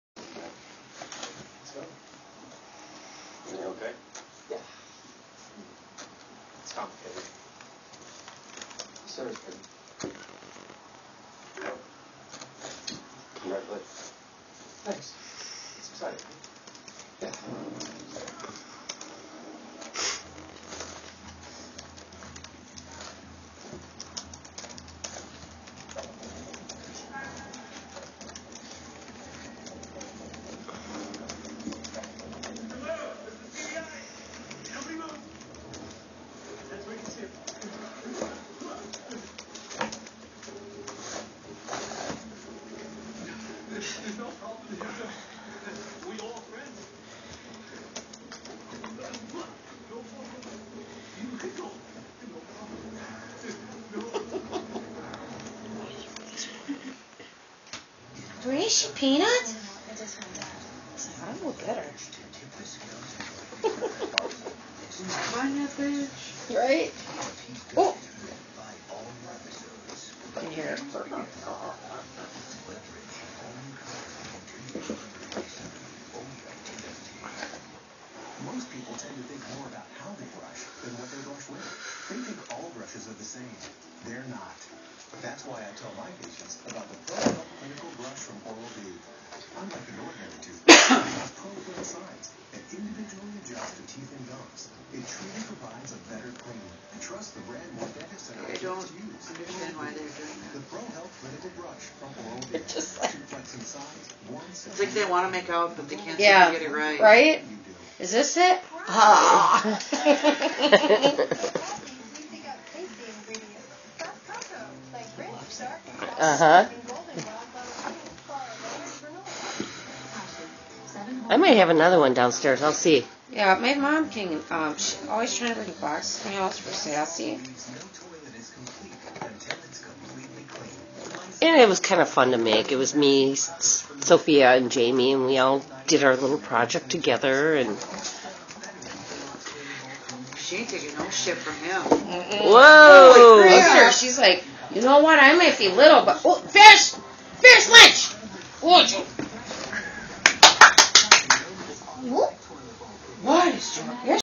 Crazy Ferrets: Peanut & Ferris's play date
Ferrets; Peanut & Ferris get together for a play date in the homemade ferret fun house. Everything starts out okay, but it turns a little aggressive towards the end.